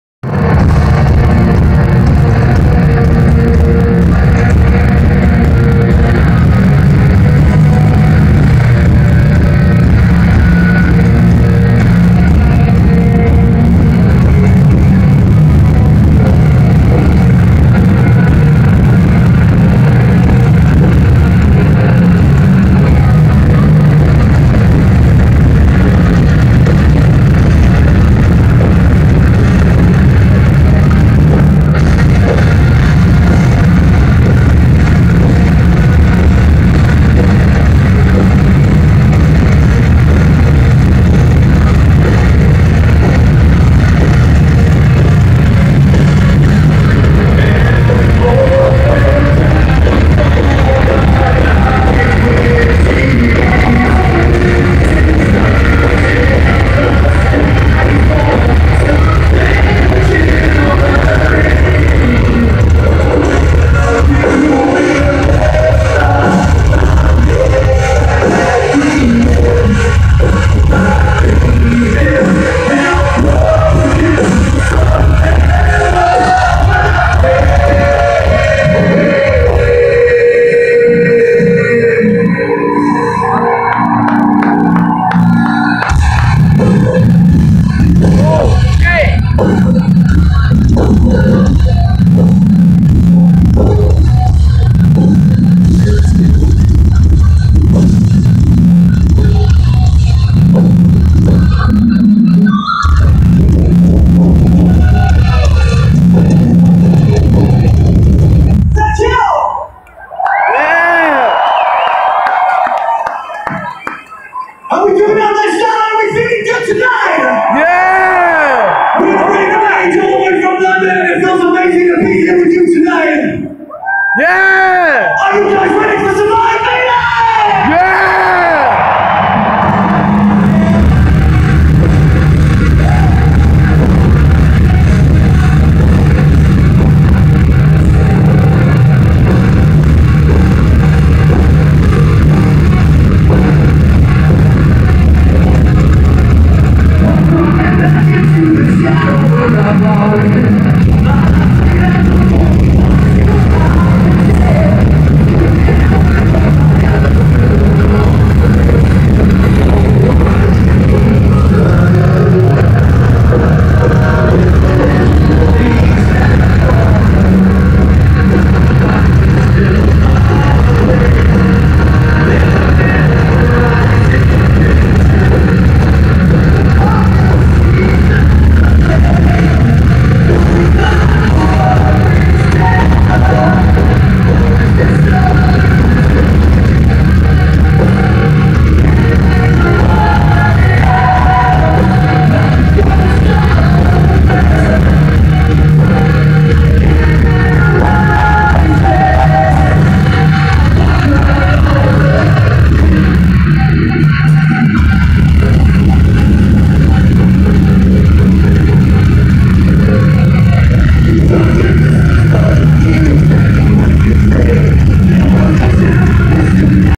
The raven age live in Charlotte nc 7/22/19
Sorry the quality is really bad the phone i have sucks but these guys are awesome go check them out there very good. Also sorry i dont know the song names since i record like half of 2 songs so sorry about that